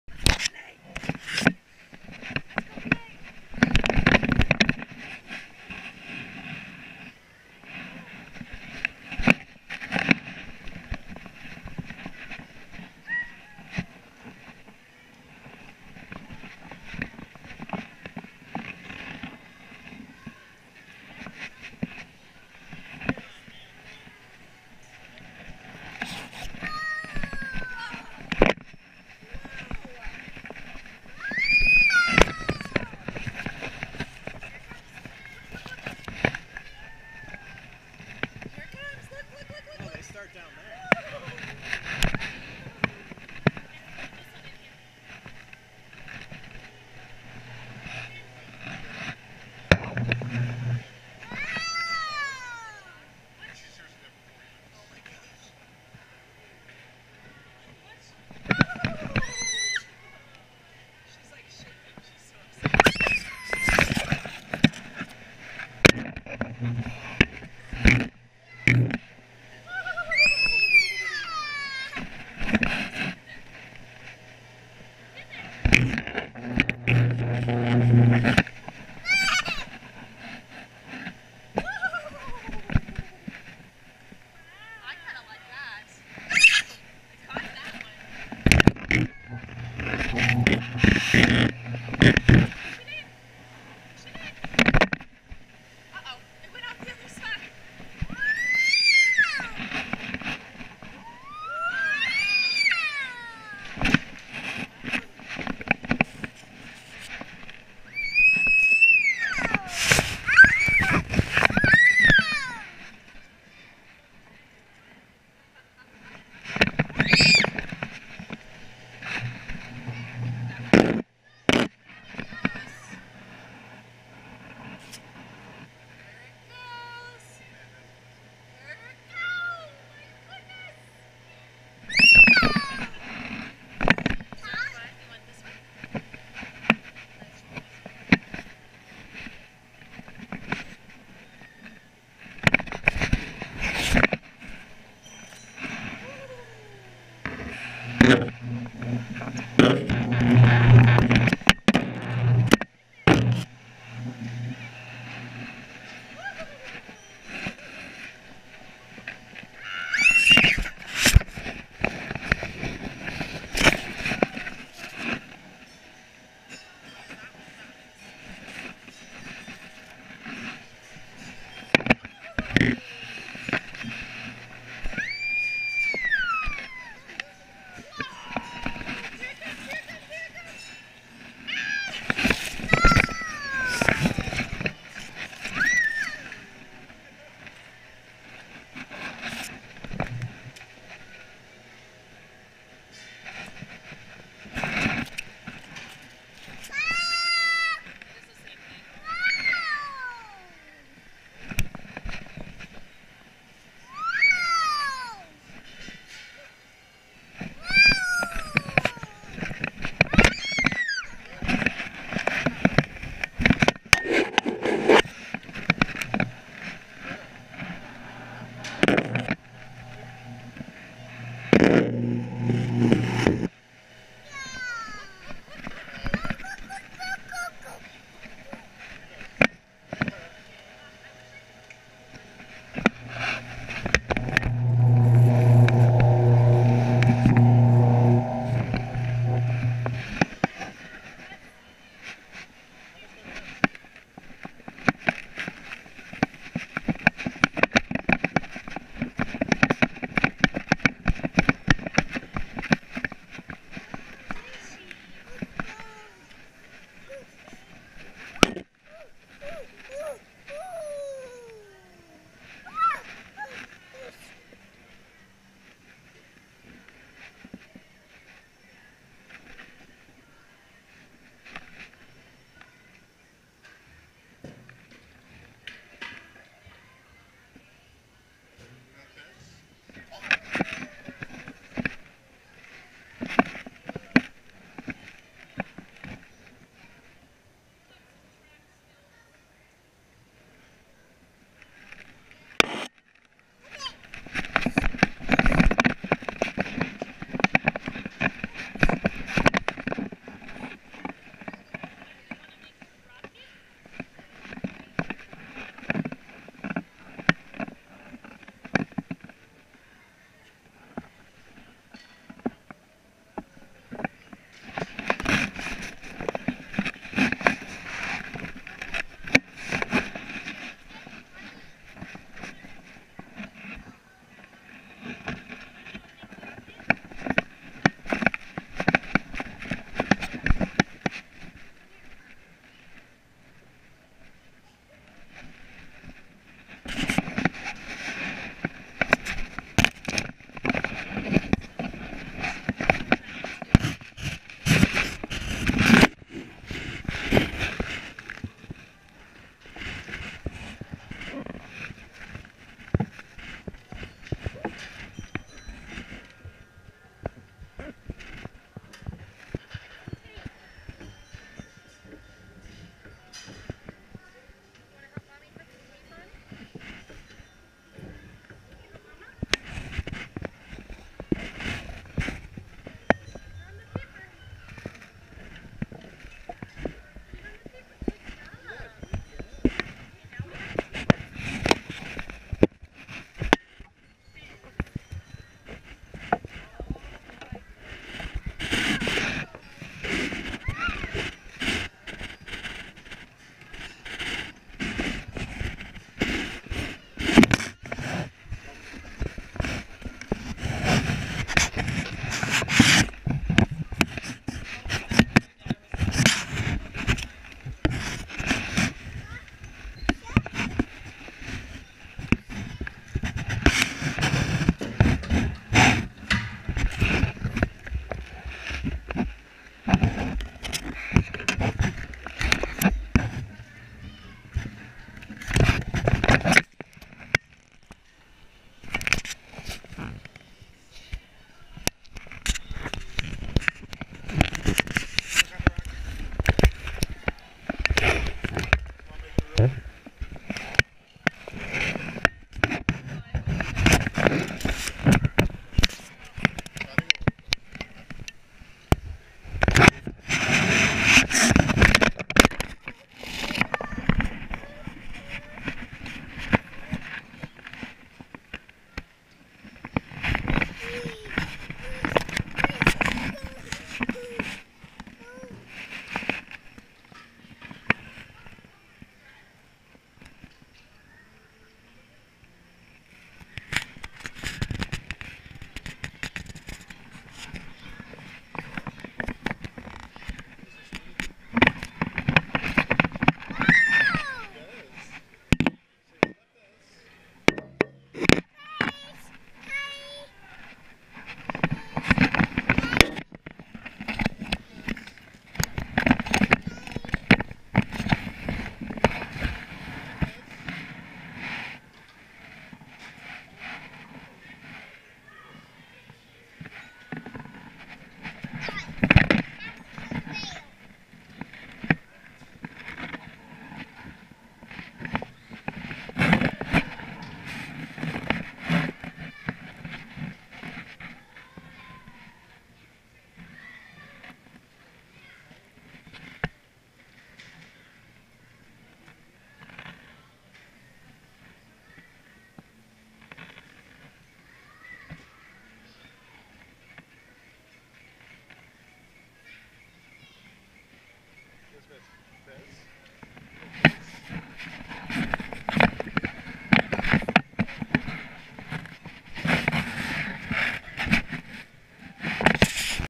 Clara's Discovery 2014
Strapped a GoPro to Baby Girl's chest while she visit the Durango Powerhouse Science Center